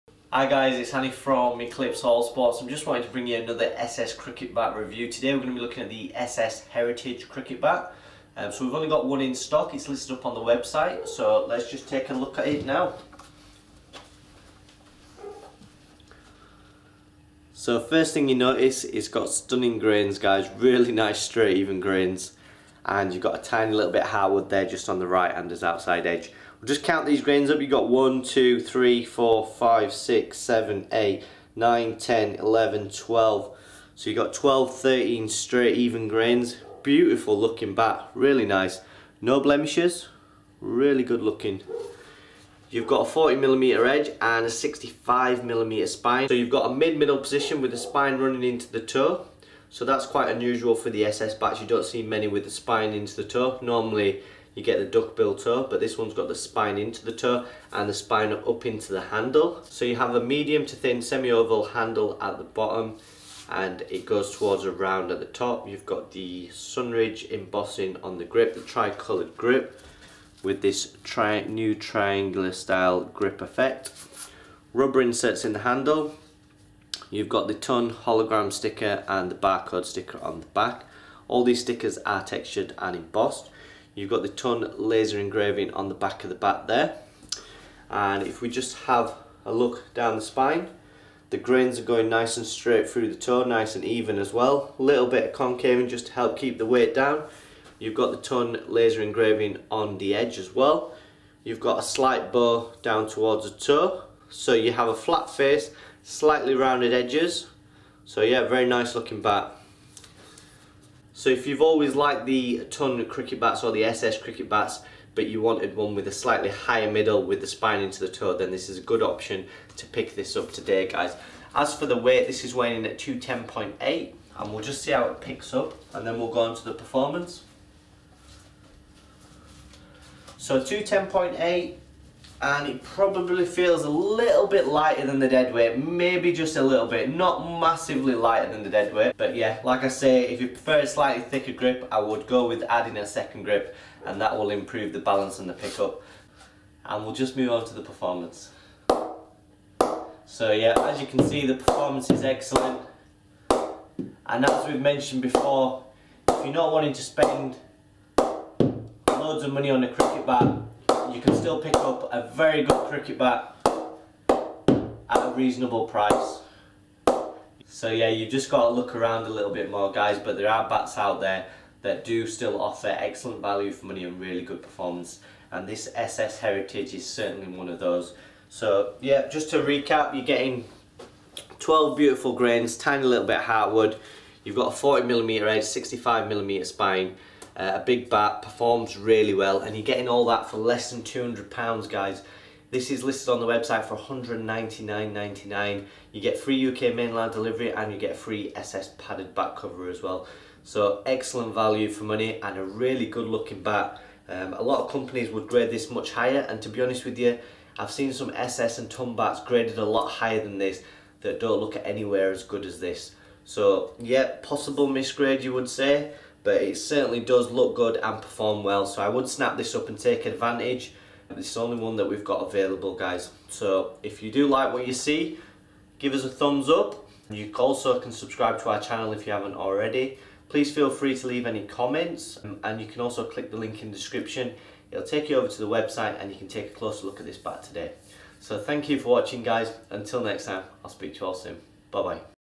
SS HERITAGE CRICKET BAT REVIEW OCTOBER 2017
Hi guys it is Hanif from eclipse all sports. I just wanted to bring you a quick video review of the new SS master heritage cricket bat.